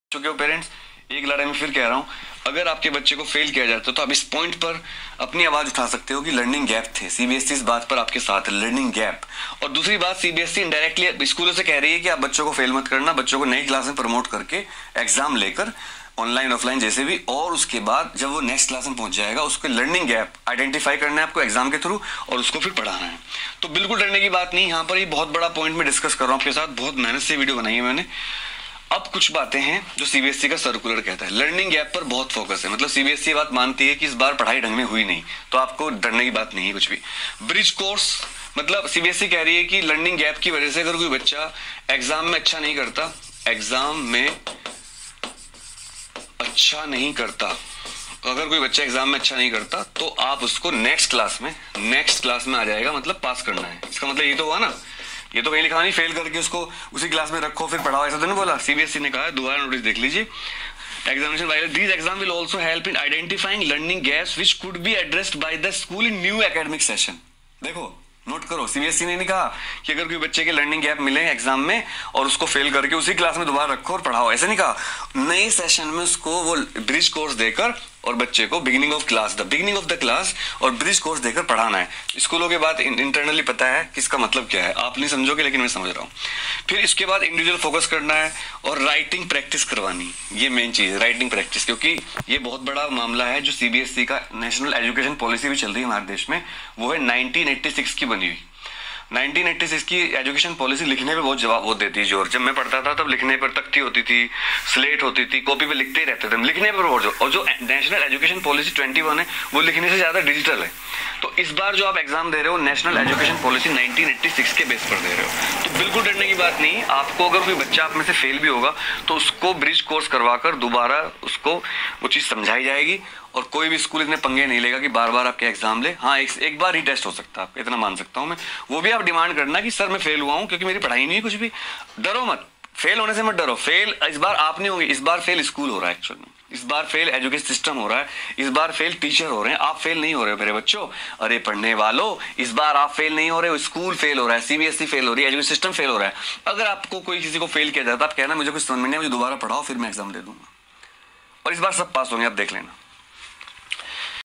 Learning Gap and Cbse Result 2021
School Fail kar de to kya kare ?
learning Gap is the Weapon to pass.
watch this master piece video by Anurag Tyagi Sir
share to all your friends